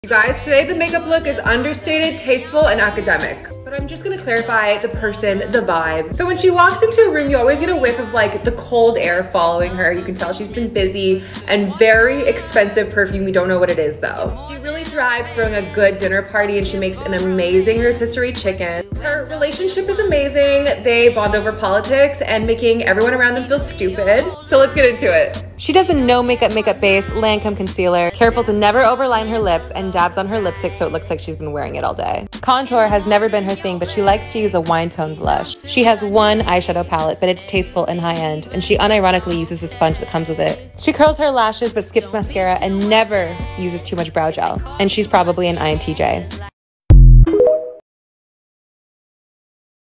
THE UNDERSTATED ACADEMIC (INTJ) MAKEUP TUTORIAL
The Intj myers Briggs personality type niche makeup tutorial, the understated academic. Inspired by the clean academia aesthetic.